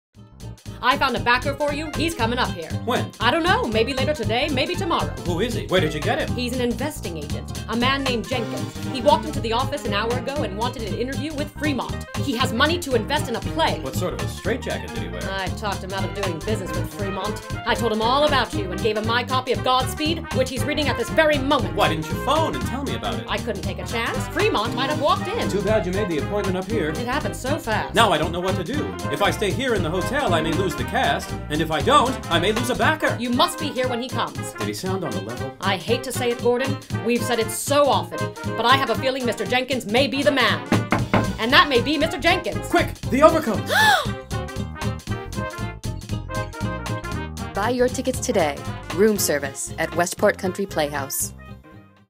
Westport Country Playhouse Presents Room Service
Room Service
By John Murray and Allen Boretz
Directed by Mark Lamos

Gordon Miller has finally discovered a formula for Broadway success: be bold, brash, and option a brilliant script by a promising young writer. What he hasn't discovered is a willing financial backer. Will the show ever go on? Never has the madcap behind-the-scenes business of putting on a play been as hilarious as it is in this classic American farce.